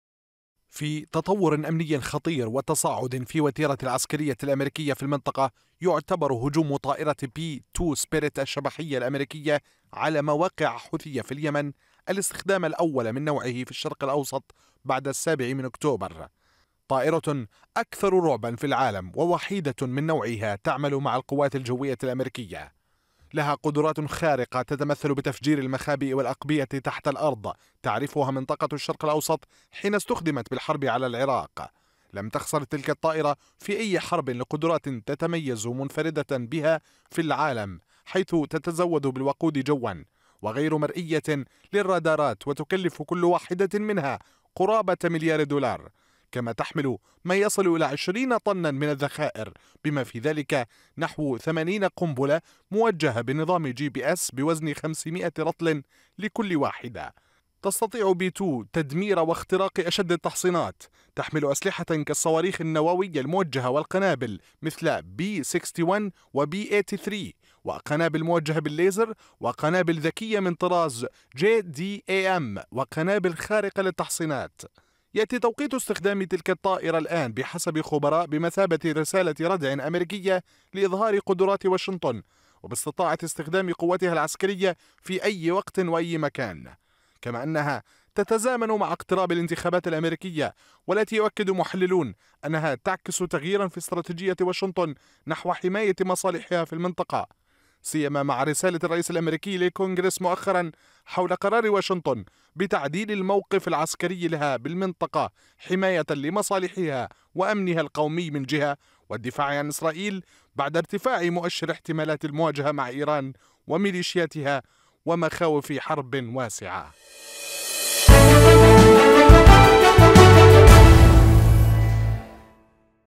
أميركا تحرك ترسانتها الأكثر رعبا وتضرب اليمن | تقرير
في تصعيدٍ لافت وتطورٍ غيرِ مسبوق في المشهد العسكري بمنطقةِ الشرق الأوسط، نفذت الولاياتُ المتحدة هجومًا جويًا باستخدامِ طائرة B-2 SPIRIT الشبحية. وهي الطائرةُ الأكثرُ تطورًا ورعبًا في الترسانةِ الجوية الأميركية.. هجومٌ يمثل رسالةً أميركيةً واضحة وردعاً عسكرياً ضد التهديداتِ المتصاعدة في المنطقة في توقيتٍ يتزامن مع ملفاتٍ حساسة أبرزُها مؤشرُ المواجهة الإسرائيلية الإيرانية.
موقع قناة التغيير الفضائية  الالكتروني